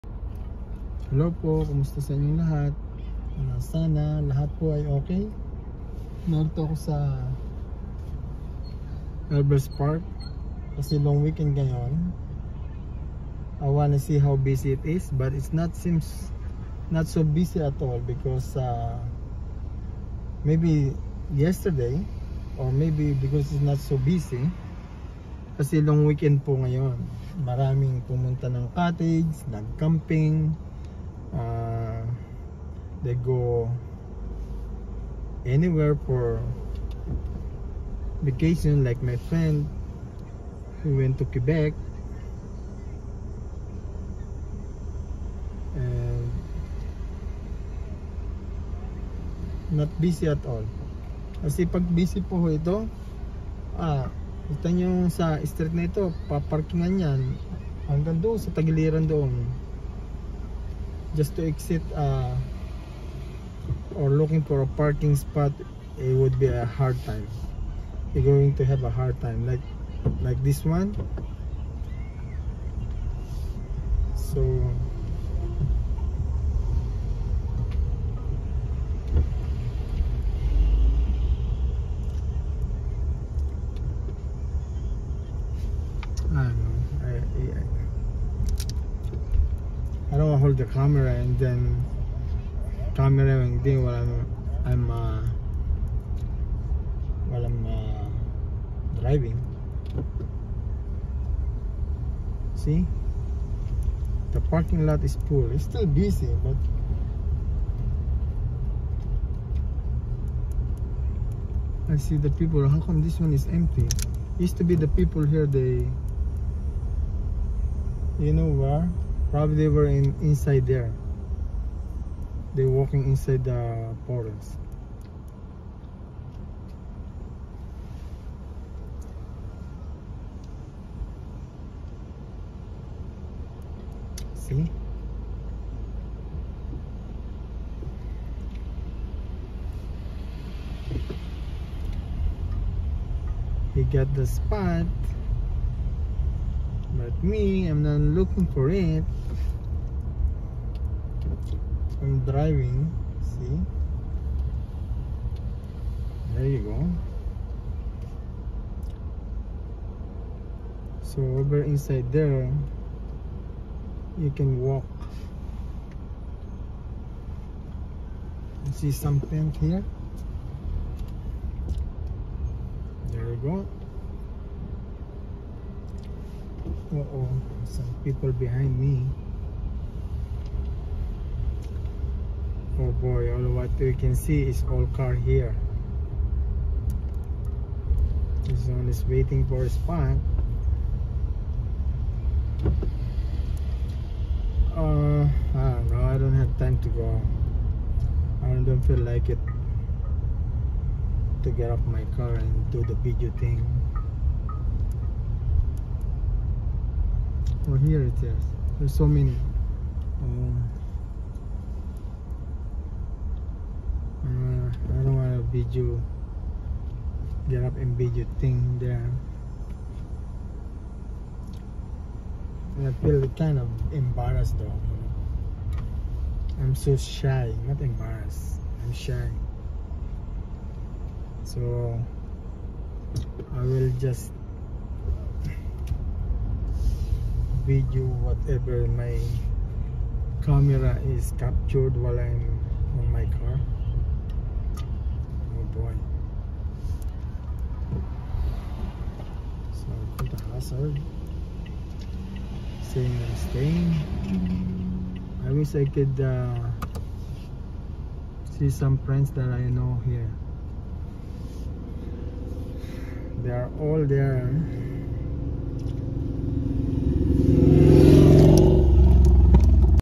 Earl Bales Park at Sunday long weekend II Canada
I came Sunday it is long weekend I wanna see if it is busy but it seems it’s not because people scattered all over the place like going camping  ,cottages and out of town .